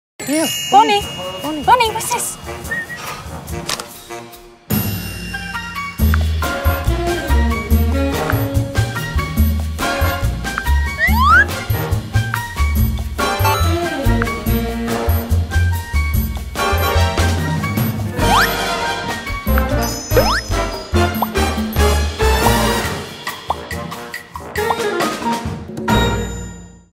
Epic Tips x Galaxy S24 Ultra: Make your pics bow WOW! | Samsung
Any pic is PAW-sible 🎨 🖼️ Move it, resize it, just like that with Generative Edit.